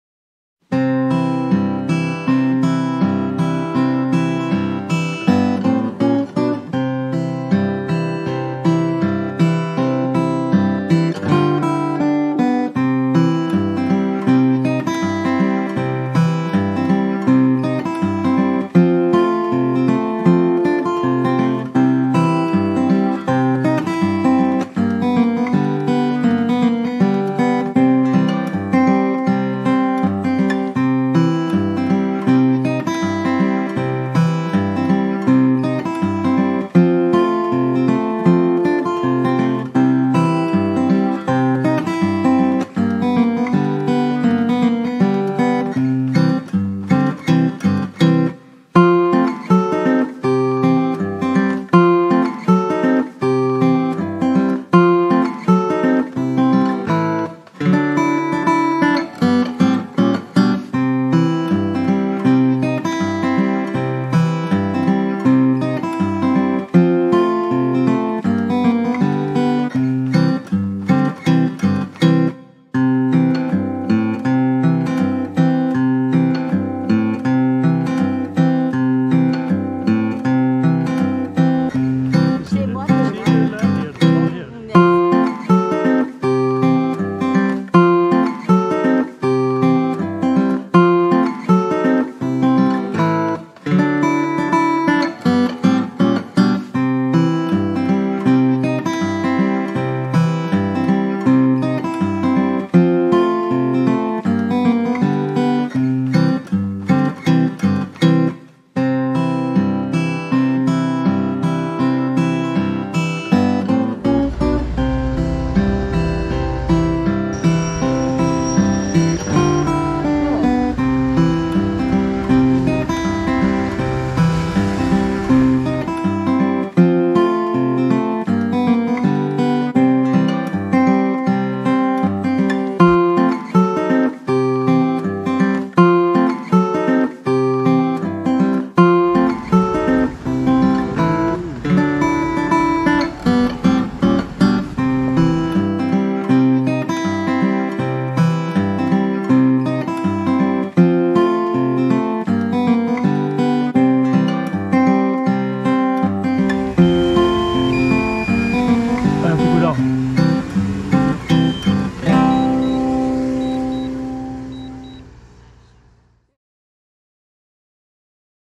Flying DJI Mini 2 drone Udonthani RC field
An evening video with nice clouds flying at Udonthani RC-field